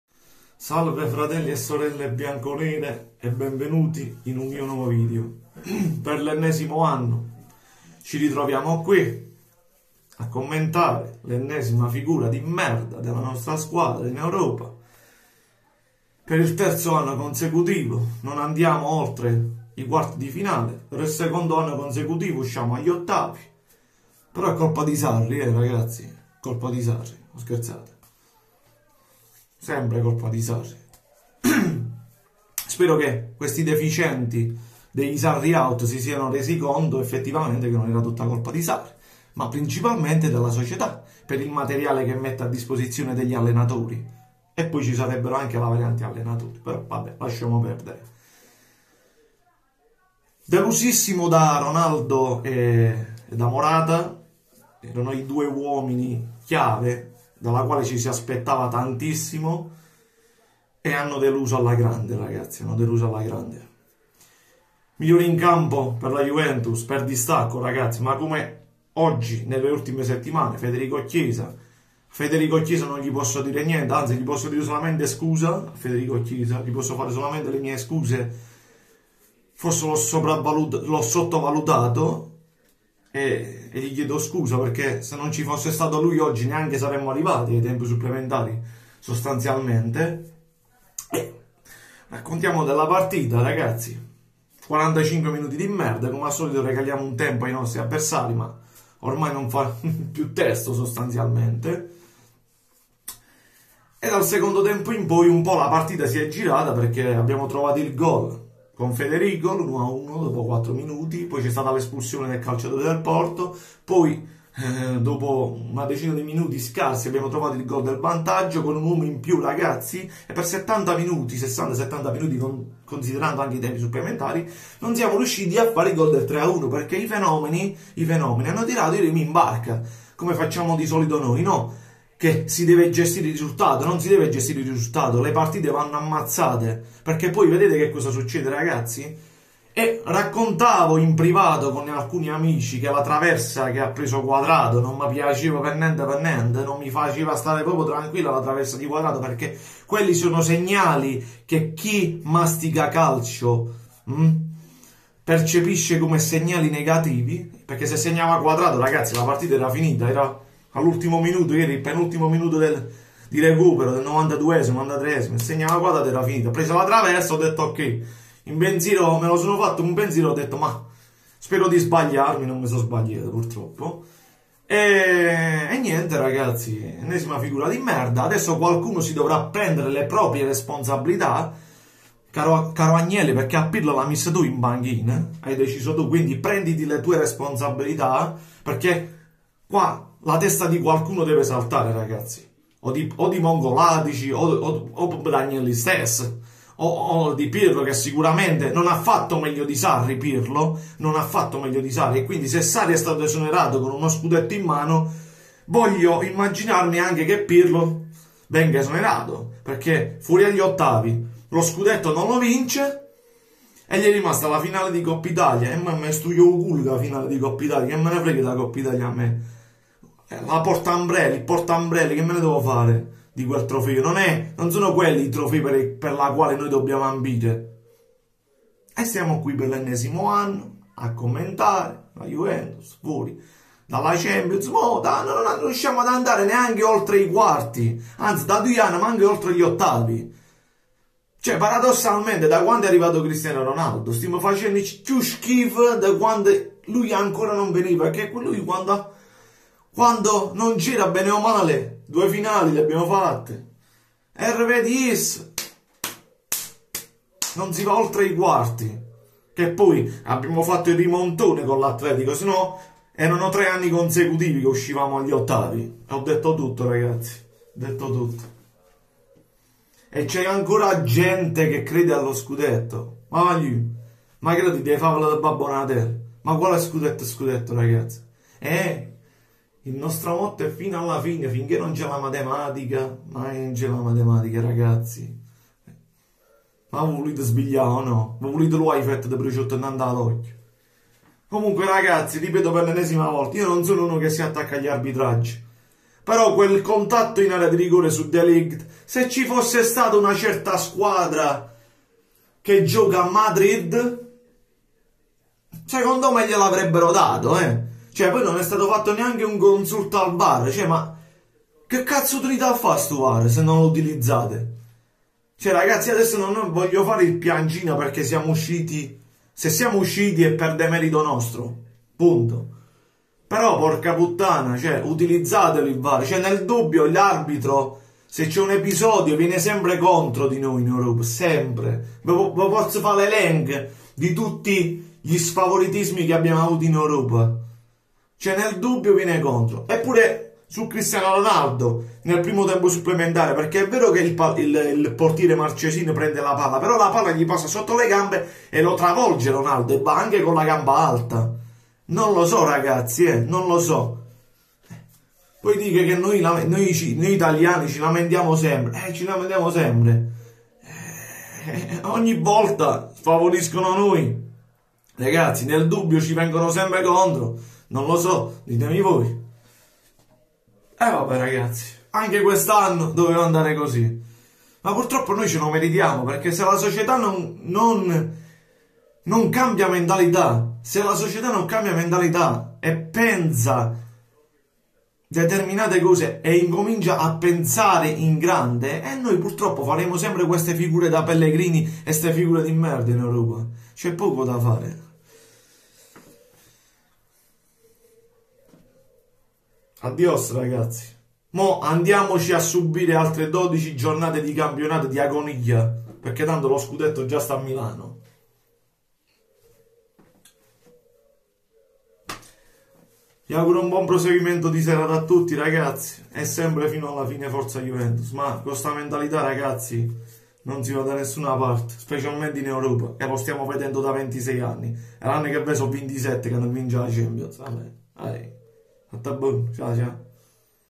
JUVENTUS 3-2 PORTO:QUALCUNO DEVE PRENDERSI LE PROPRIE RESPONSABILITÀ.VERGOGNATEVI 😡😡😡
Ennesimo fallimento Europeo per la Juventus